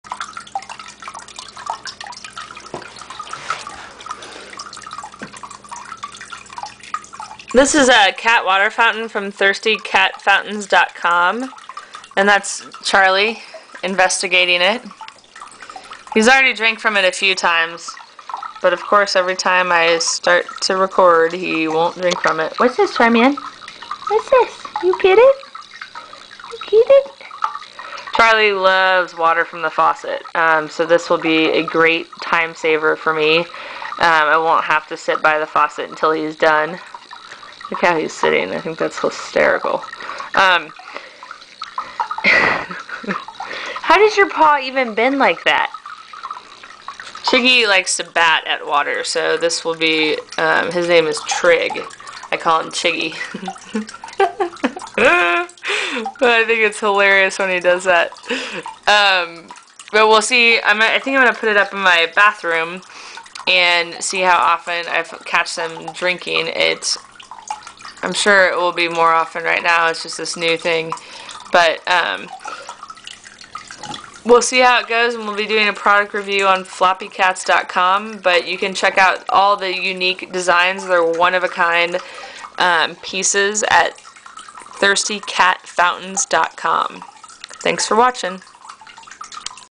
Cat Water Fountain - Pet Fountains by Keith Davitt - ねこ - ラグドール - Floppycats
This is Charlie, a 1-year old Seal Mitted Ragdoll Cat, investigating the Thirsty Cat Fountain by Keith Davitt

Thinking of adopting a Ragdoll cat? Check out our book A Ragdoll Kitten Care Guide: Bringing Your Ragdoll Kitten Home or watch our video on YouTube